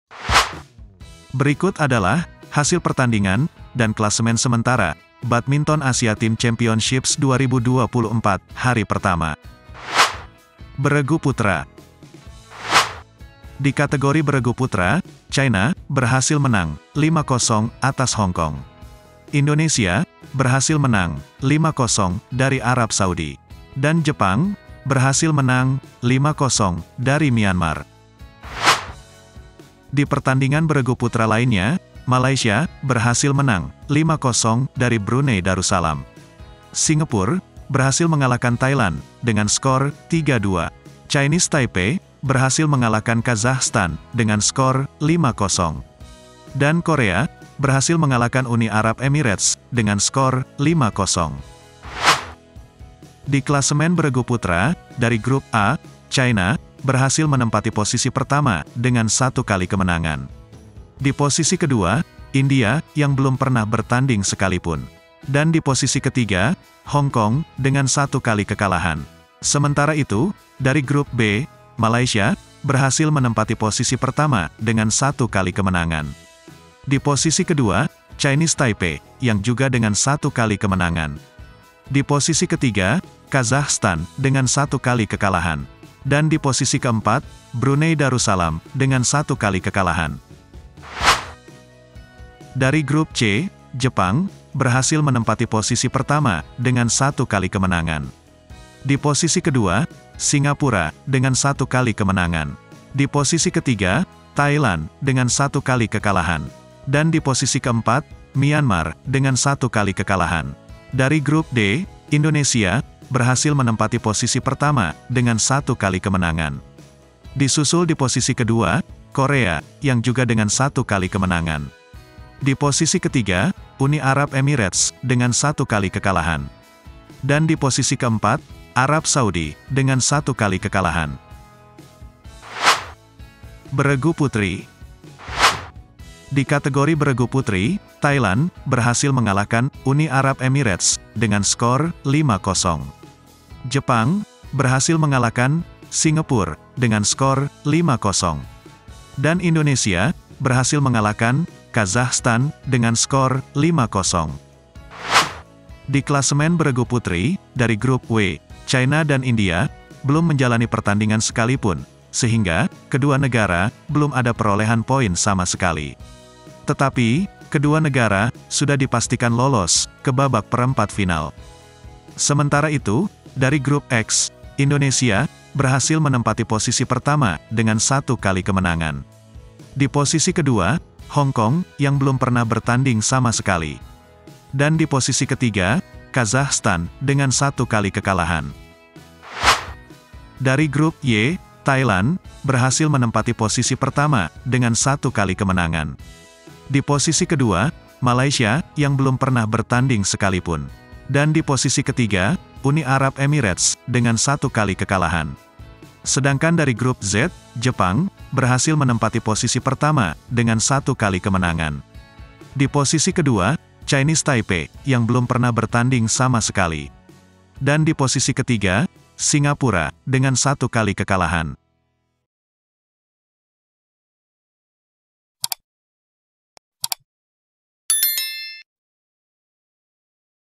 Hasil & Klasemen Sementara Badminton Asia Team Championships 2024 Day 1. Indonesia Di Puncak
Klasemen Sementara Badminton Asia Team Championships 2024 Day 1. Tim Beregu Putra & Putri Indonesia Di Puncak Klasemen